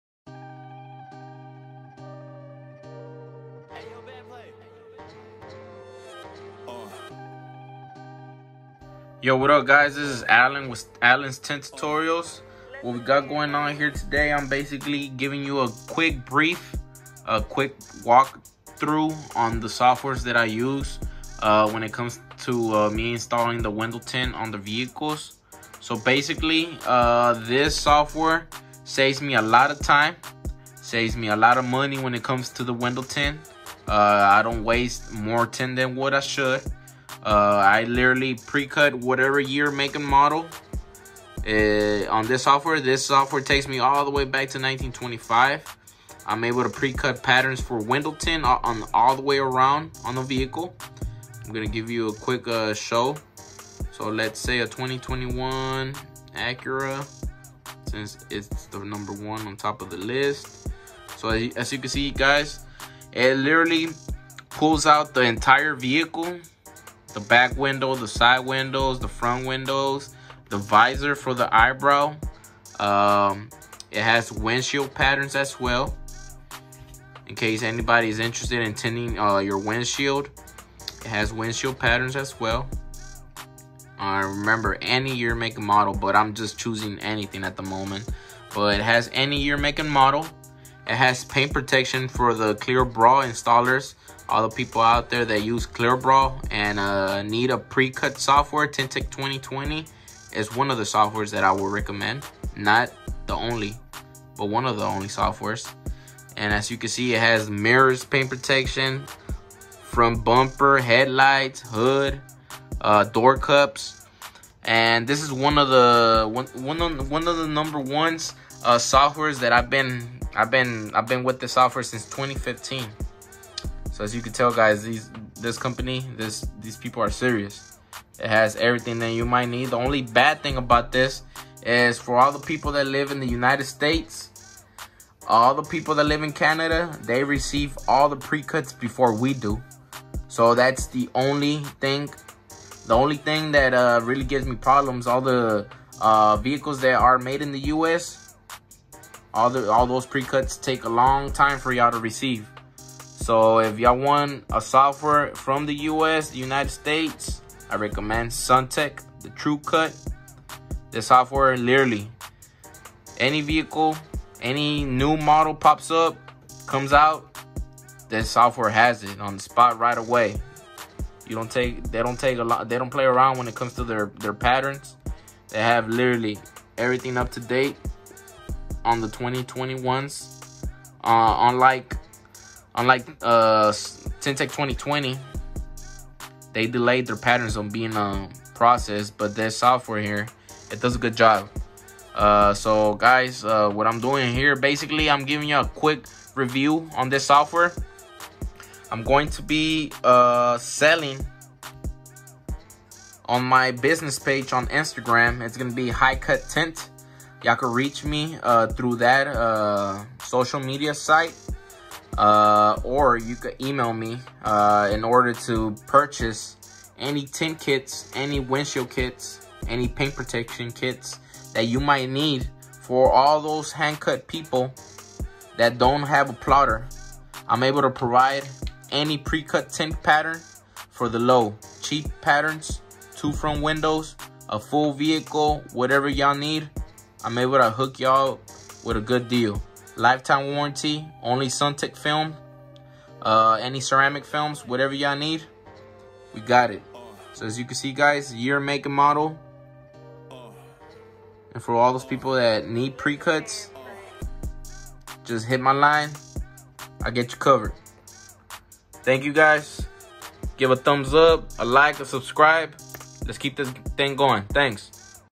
What Window Tint SoftWare You Should Use ❓
What Window Tint software Is Best For Your Precut Tint Patterns ?
Tint TEK 2020 ? Or Suntek TrueCut?